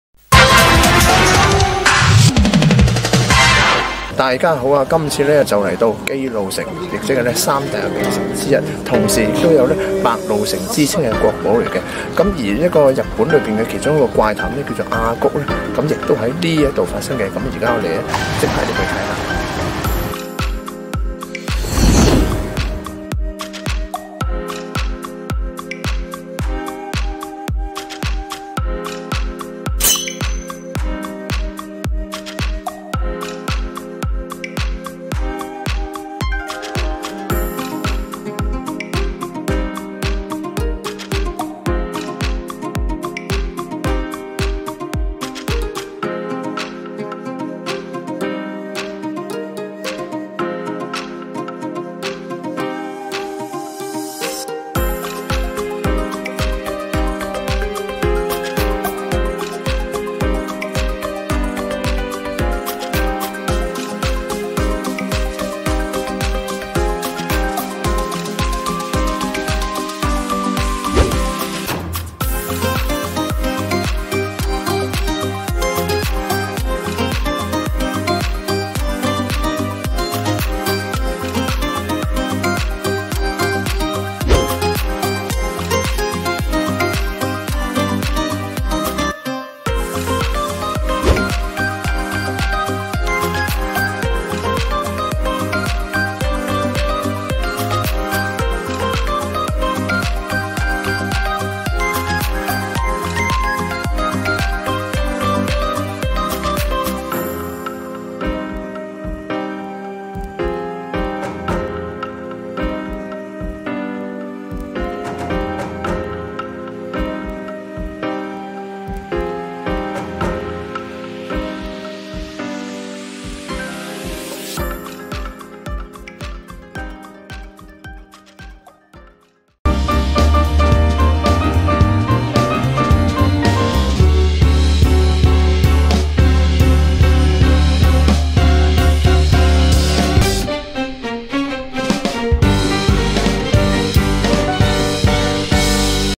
【日本旅遊】日本三大名城之姬路城/內部參觀/怪談:數盤子的阿菊
Non 2019
日本三大名城之姬路城